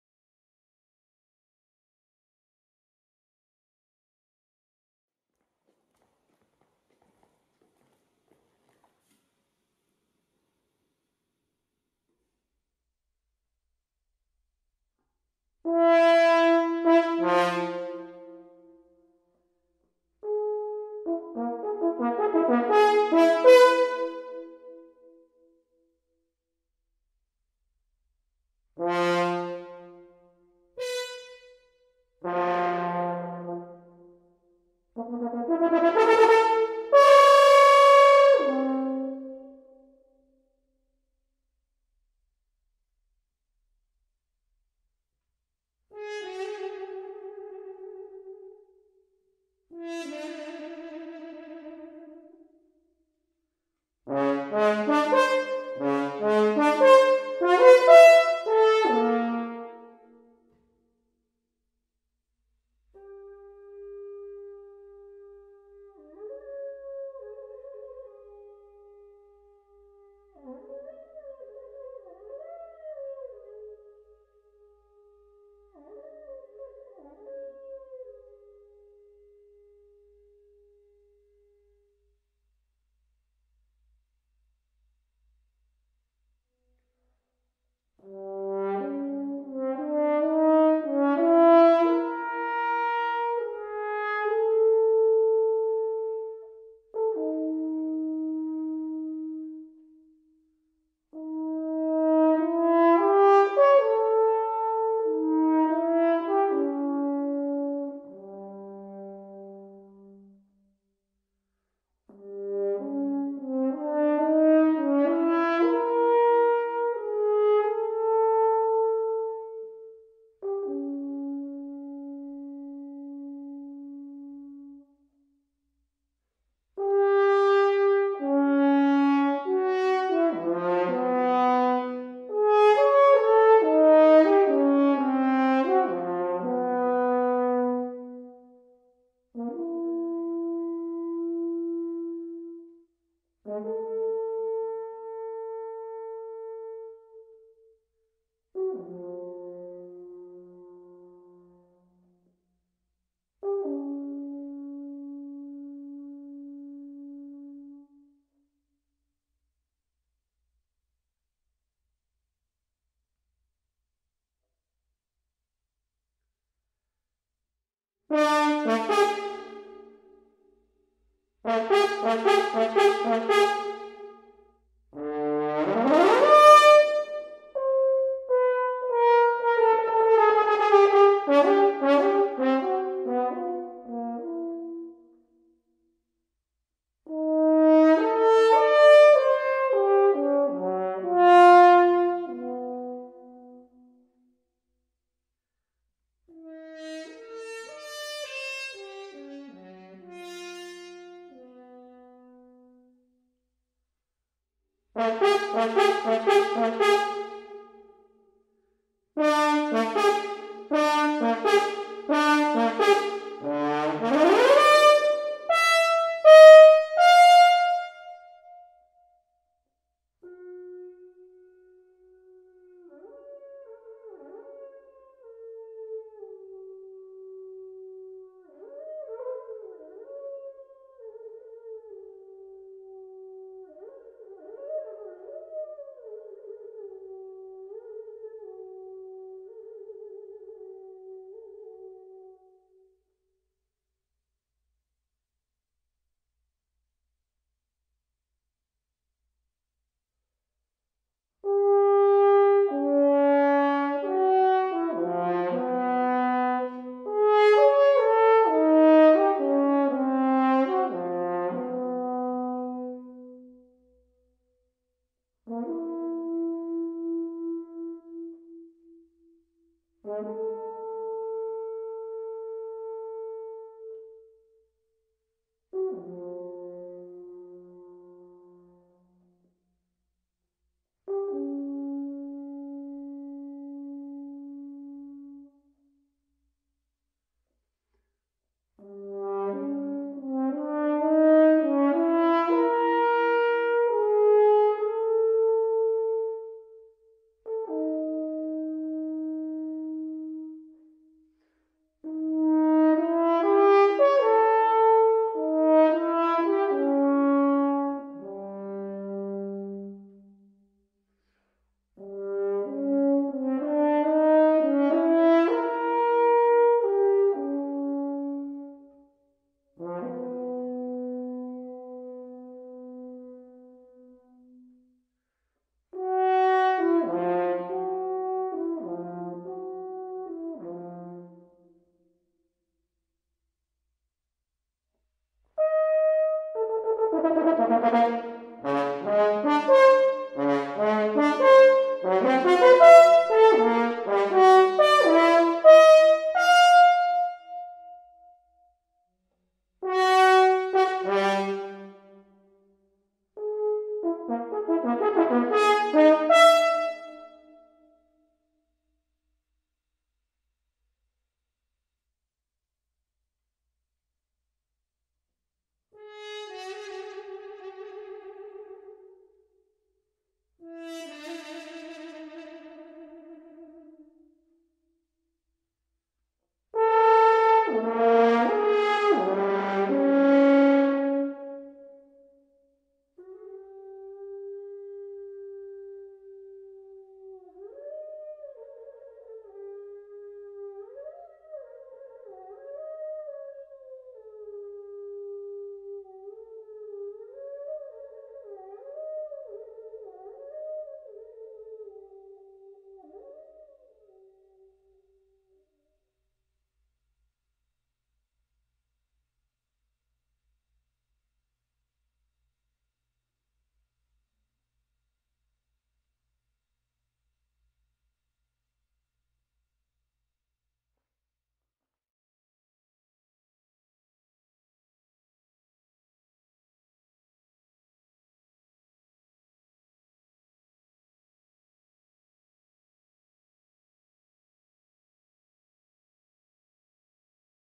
DEMO MICROS A.E.A. - Solo cor
Le R88 a été optimisé pour l’enregistrement des instruments et des ensembles stéréo. Les deux moteurs à ruban du R88 sont orientés à 90 degrés, et offrent une excellente réponse hors-axe.

Le R88 utilise la configuration à ruban de grande taille de la gamme des micros RCA 44, ce qui donne au micro une solide réponse dans les basses jusqu’à 20 Hz et une douceur extrême dans les hautes fréquences, très flatteuse pour les sonorités complexes comme les cymbales, les cordes et les bois.

Référence A.E.A. utilisée pour l'enregistrement
- Ambiance pièce : R88